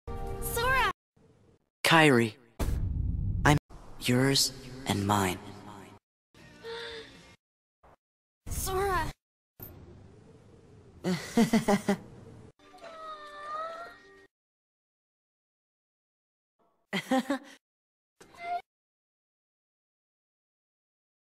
Sora and Kairi I'm Yours and Mine ,,2
Sora Dialogue Haley Joel Osment
Kairi Dialogue Hayden Panettiere
Voice Actors
Sora by RSunderland
Kairi by MayCakes
Christmas Town Kazuki9484
Sora hold Kairi are romances in Christmas Town
Disney and Square Enix